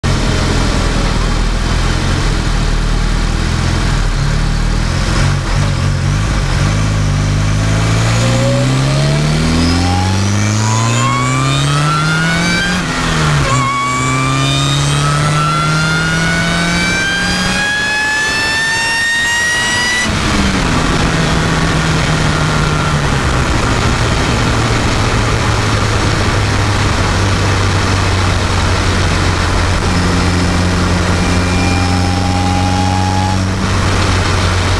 Cobalt ss with tvs supercharger sounds
Microphone by the intake inside the engine bay. With a 2.9 pulley on a tvs 1320. Slow pull to capture the sweet sounds of the tvs